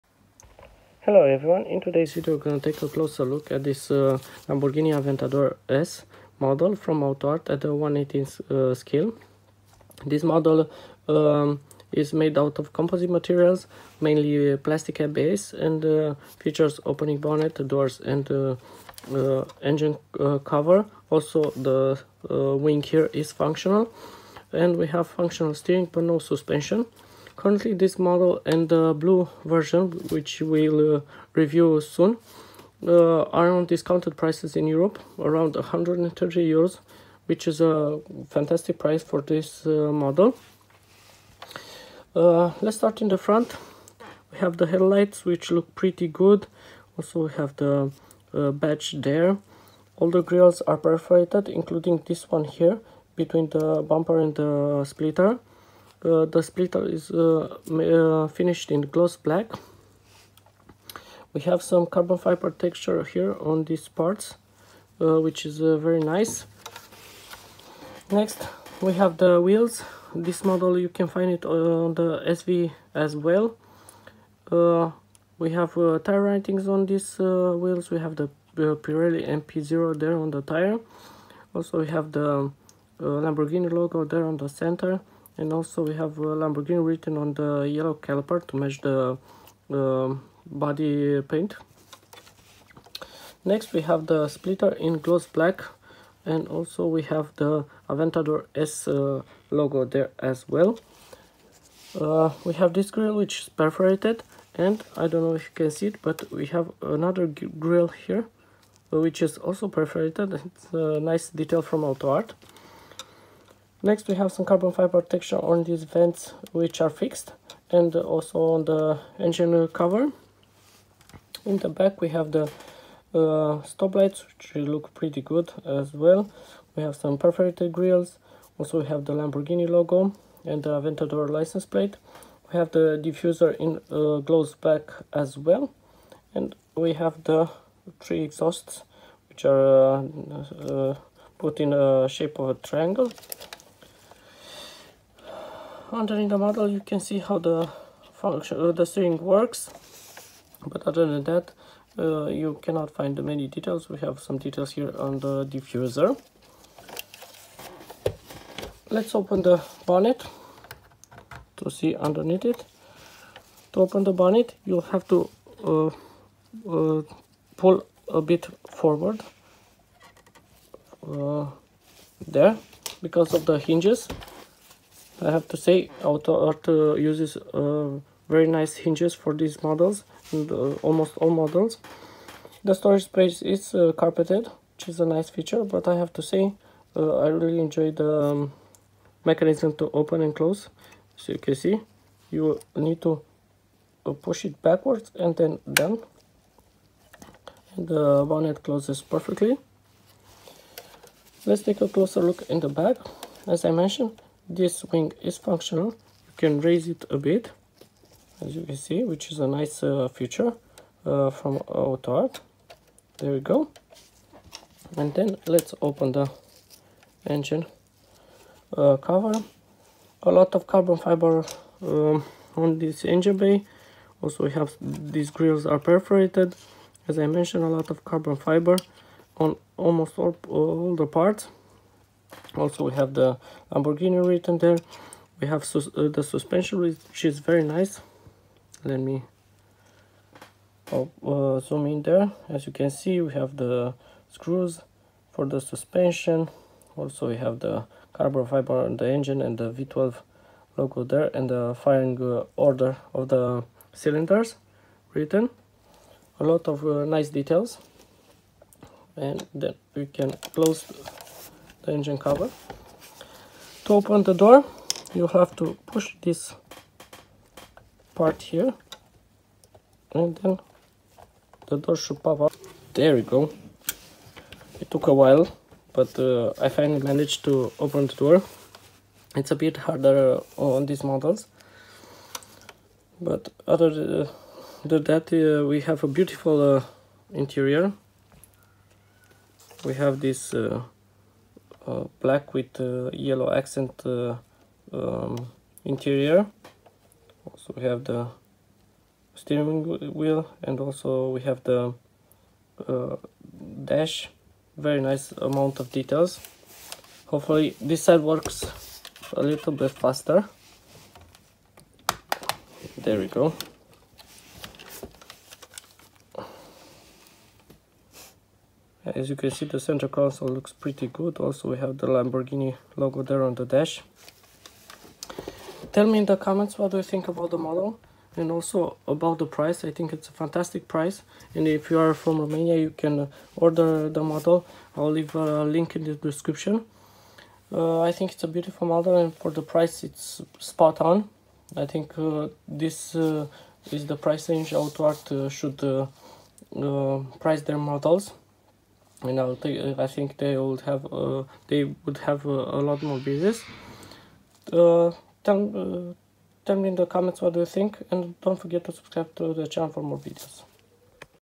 Unbelievable Deal: Lamborghini Aventador S (2017) 1:18 AutoArt - Just €130
Get ready for an exclusive opportunity! Join us as we unveil the incredible deal on the Lamborghini Aventador S (2017) 1:18 by AutoArt, now available at the jaw-dropping price of just €130 In this detailed review, we explore why this offer is too good to pass up. Discover the exceptional craftsmanship, attention to detail, and the prestigious design that AutoArt brings to this iconic supercar replica. Whether you're a seasoned collector, a Lamborghini enthusiast, or someone looking to kickstart a diecast collection, this review will provide an in-depth look at what makes this Aventador S model a steal at such an affordable price. Don't miss out - watch now and secure your piece of automotive excellence!